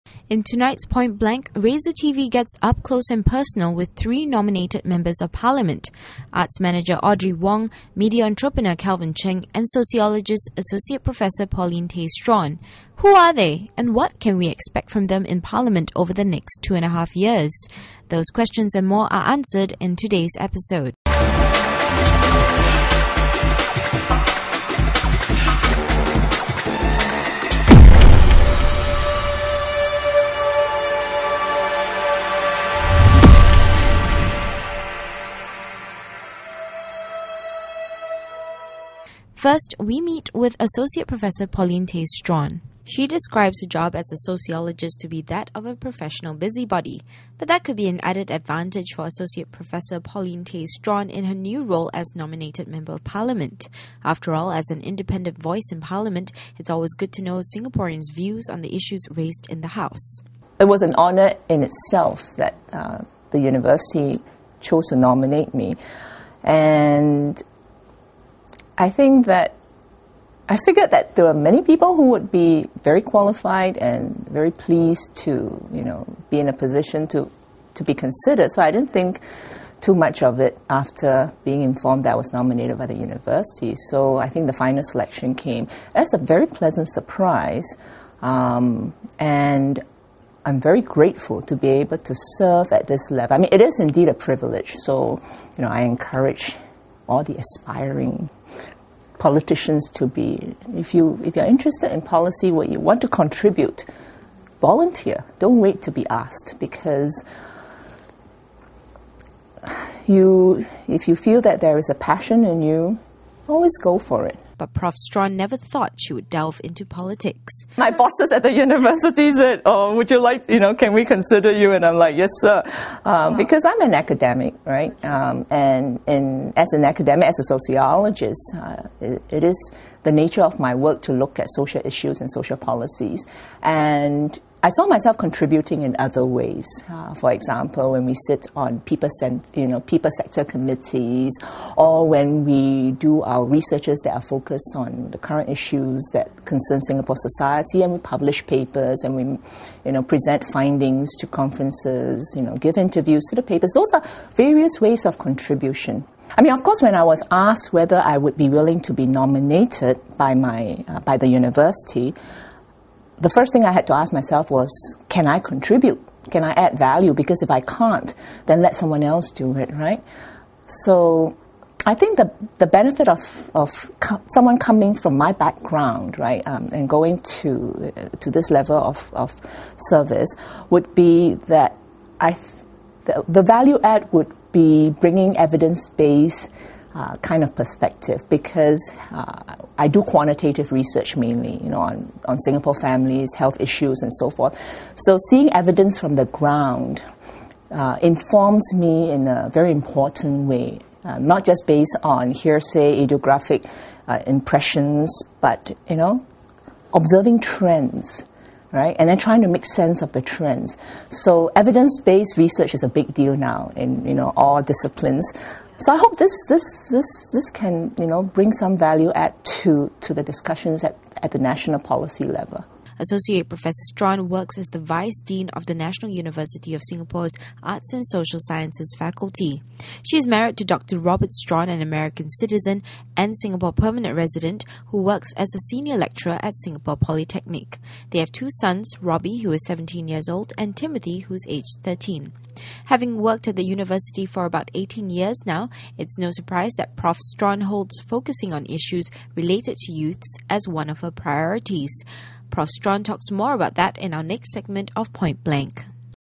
"It's a privilege to be an NMP" (Pt 1 NMPs in focus)
We talk to three new Nominated Members of Parliament - arts manager Audrey Wong, media entrepreneur Calvin Cheng, and sociologist Associate Professor Paulin Tay Straughan. But in this first segment, we find out from Prof Straughan about how she feels becoming an NMP, and why she never thought about delving into politics before.